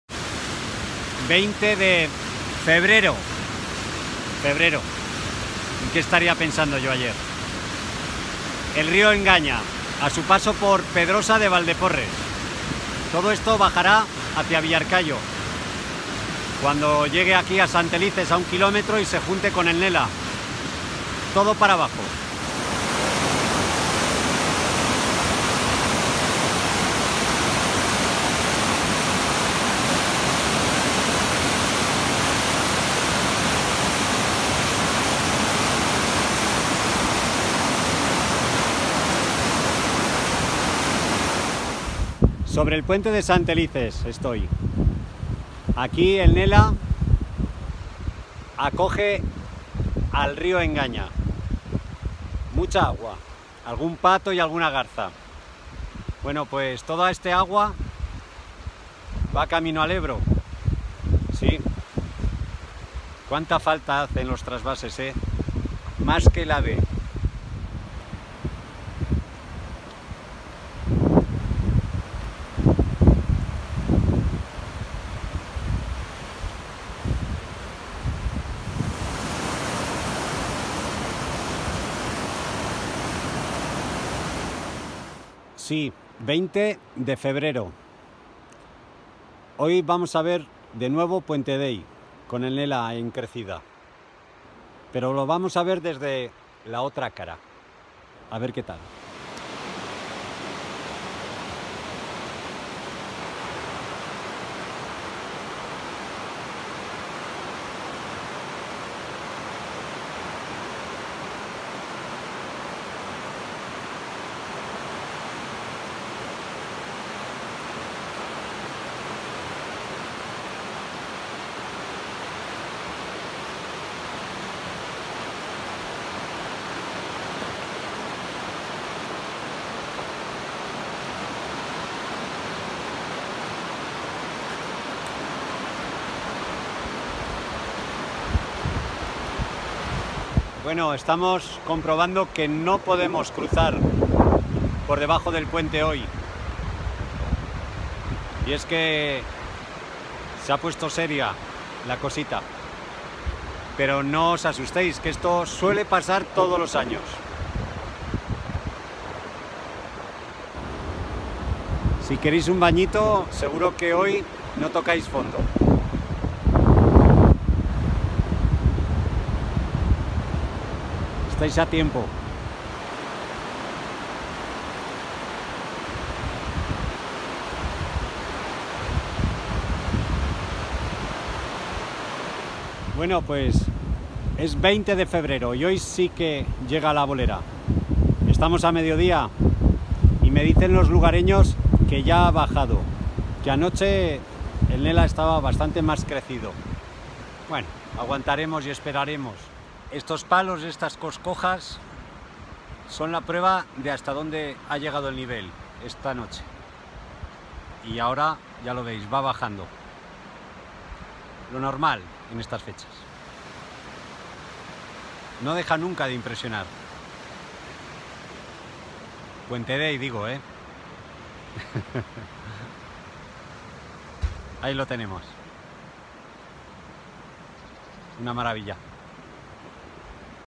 Recorrido en la crecida del Río Nela
Recorremos la cuenca alta del Nela comprobando su caudal.